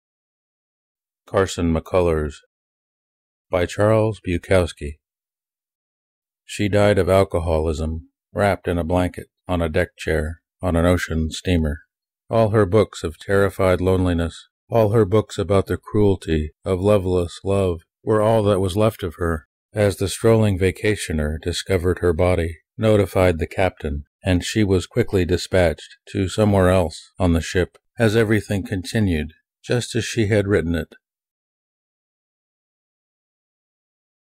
"Carson McCullers," by Charles Bukowski
poem--McCullers: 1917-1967. Bukowski: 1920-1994. Image of McCullers from LIFE magazine online archive. Images of Bukowski and steam ship from wikimedia, grateful acknowledgement. Poem copyright Estate of Bukowski.